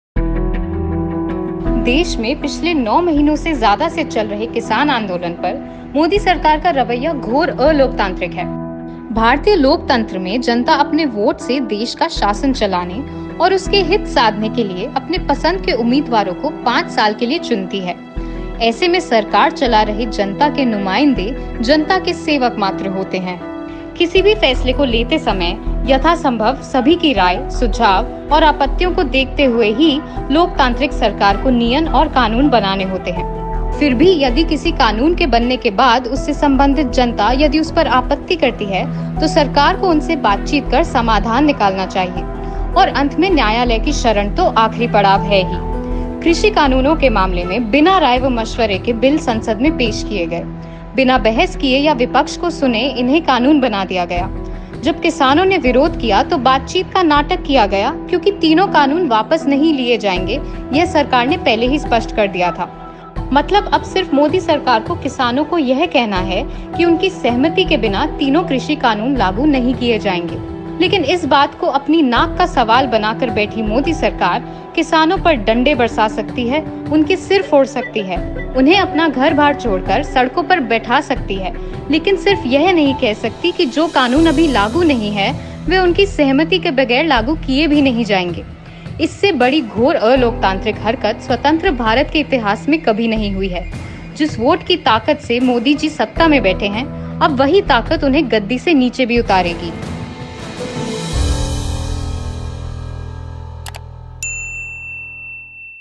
अन्नदाता की एक ही तो मांग है- बीजेपी सरकार काले कानून रद्द करे
बीजेपी सरकार देश के अन्नदाता के आक्रोश को अनदेखा और आवाज को अनसुना कर रही है। लेकिन बीजेपी सरकार अहंकार में न रहे, उसको अन्नदाता की मांगों के सामने झुकना ही पड़ेगा।